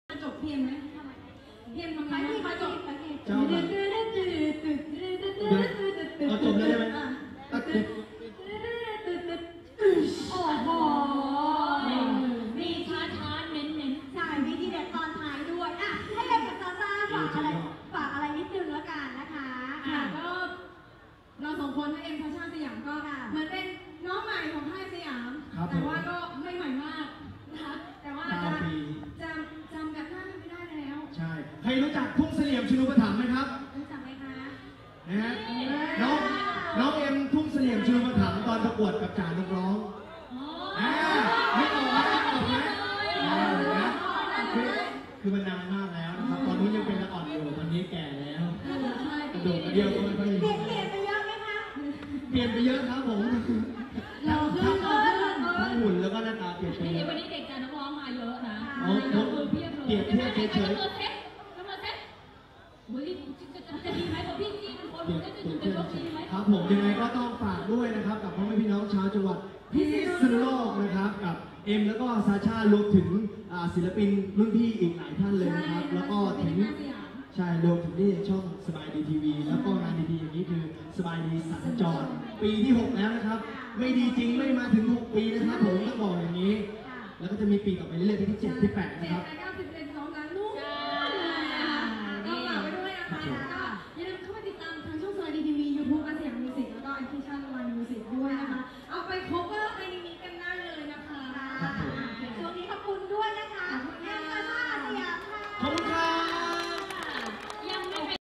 พิธีกร VJ-ต๊อ-มิ้น-เอ็ม-ชาช่า อาร์ สยาม-Hot Thai singer
คอนเสิร์ตสบายดีสัญจร ปี 6 พิษณุโลก 12 กันยายน 2558 หน้าบิ๊กซี